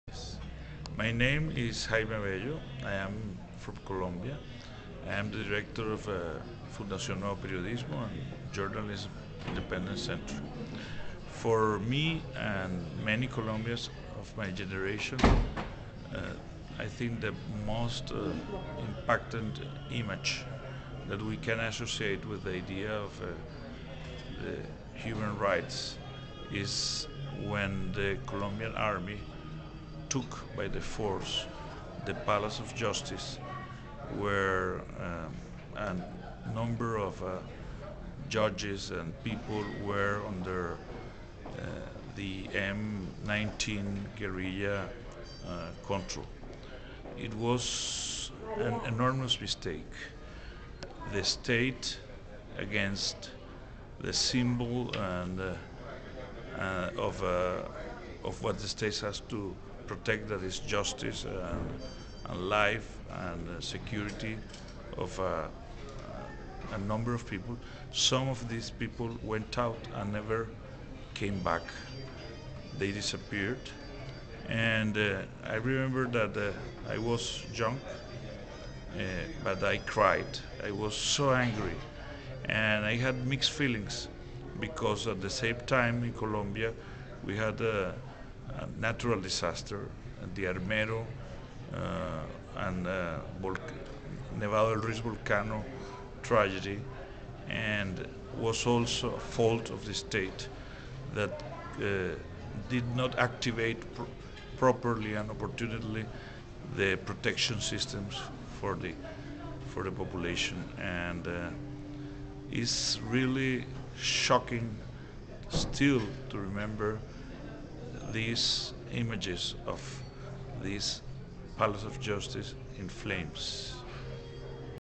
Jaime Abello Banfi Interview (English version) - December 10th, 2008
Interview with Jaime Abello Banfi, Director of the Fundacion Nuevo Periodismo Iberamericano, a Colombian journalism training organisation. Interviewed at the GFMD's Athens conference as part of WITNESS' UDHR60 campaign.

He answers our question, "What image opened your eyes to human rights?" by talking about the image seared in the minds of an entire generation of Colombians - the storming of the Palace of Justice in November 1985.